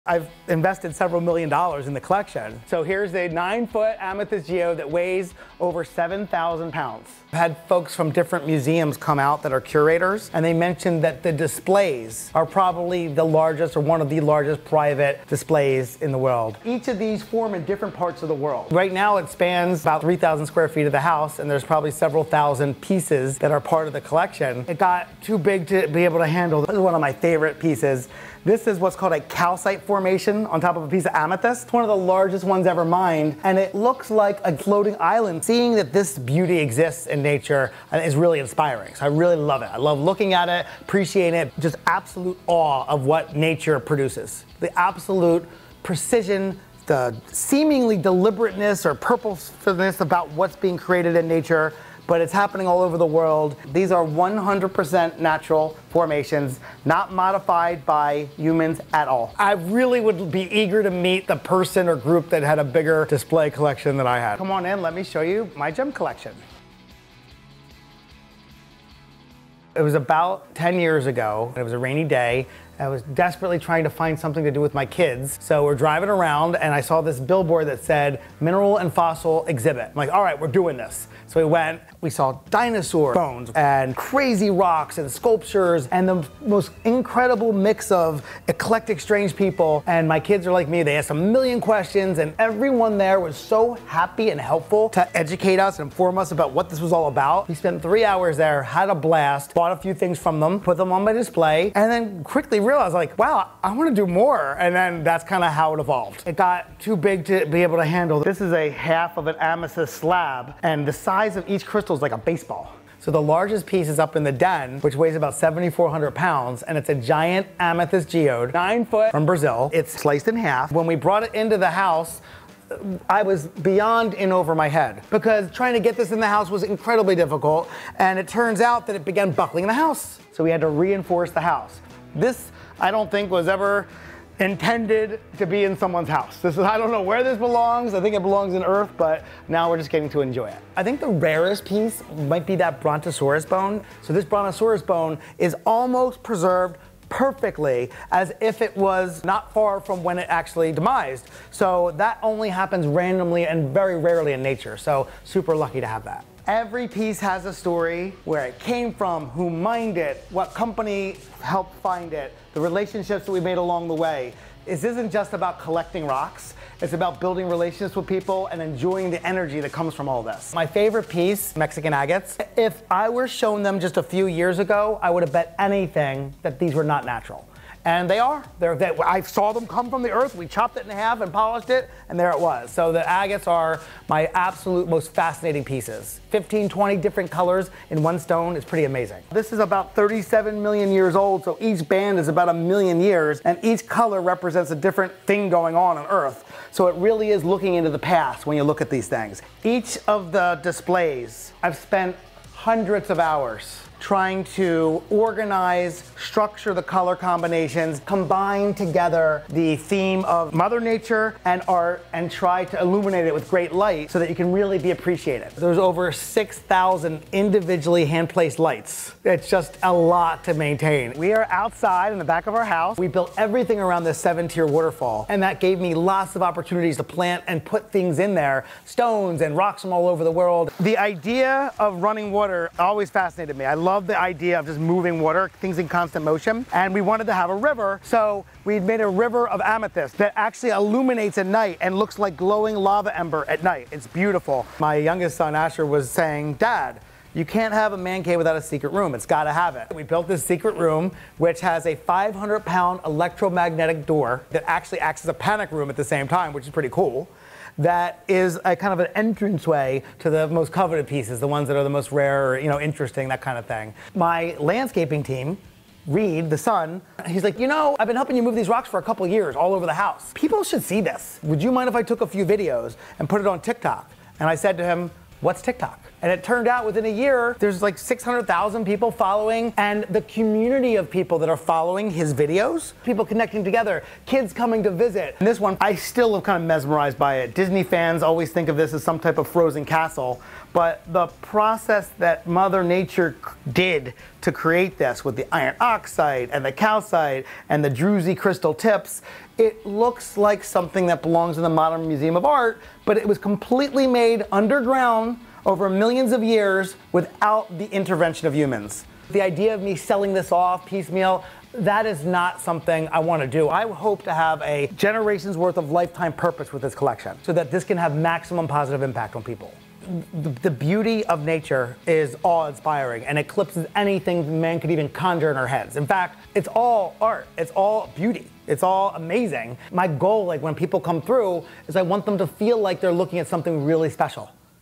This man's gem collection may be the world's best private display
He's known as The Gem Guy on tiktok and his videos about his massive geodes and secret rooms full of gems have amassed him over 500k followers on the platform.

The Gem Guy began collecting gems after taking his kids to a gem exhibition nearly 10 years ago and his fascination grew. "Seeing that this beauty exists in nature is really inspiring. I really love it".

From there his collection expanded with a giant amethyst geode that weighs over 7400 pounds. "When we brought it into the house...it started buckling the house, so we had to reinforce the house."

The backyard has a 7-foot waterfall surrounded by an amethyst river and behind a 500-pound door is a secret room with his most coveted pieces.

"The idea of me selling this off piecemeal, that is not something I was to do. I hope to have a generation's worth of purpose with this collection. I want this to have maximum positive impact on people."